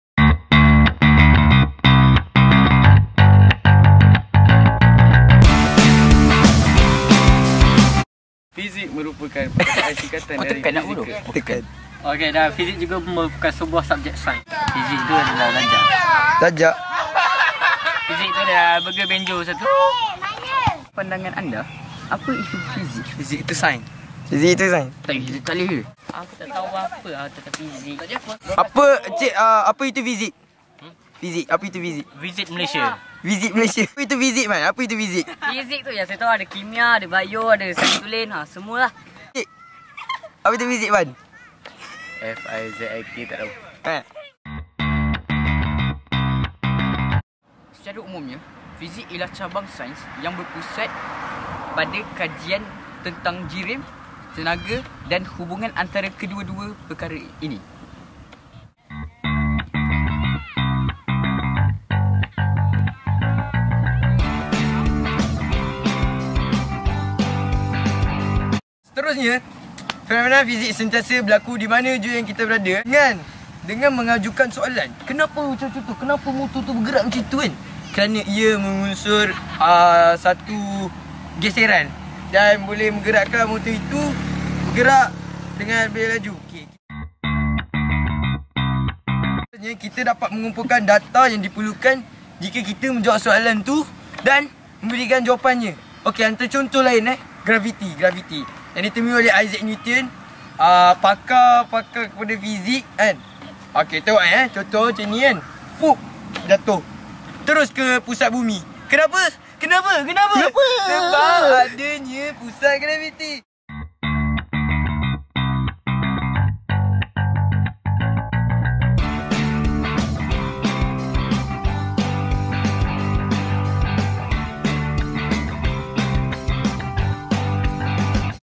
Interview Fizik oleh Aiman Halim dan Azrul SMK Cyberjaya
Tugasan 1: Apa itu Fizik?
January 2018,
SMK Cyberjaya.